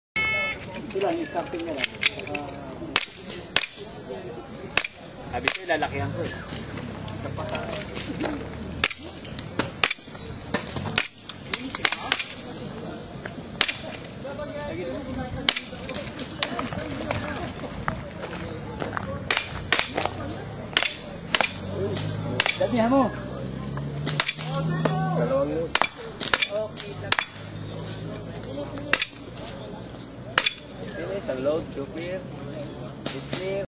The 5.11 CHALLENGE: 1st Ever IPSC ASIAN Level III ACTION AIR OPEN and INTERNATIONAL CHAMPIONSHIPS
NROI:
  OPSA Firing Range, Subic Bay, Olongapo, Philippines.
3-5 June 2011